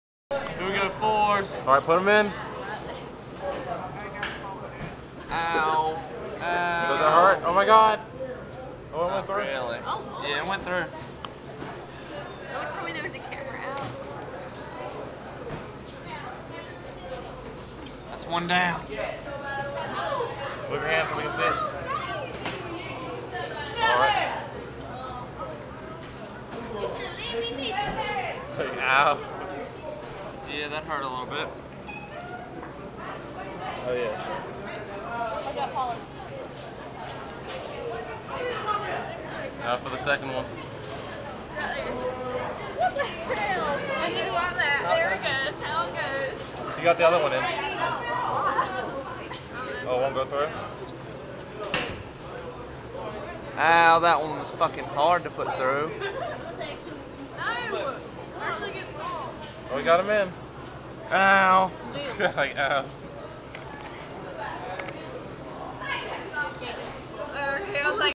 Tyler stretches to a 4g
tyler gauging or as smart ass pricks say "stretching" his ear to a four!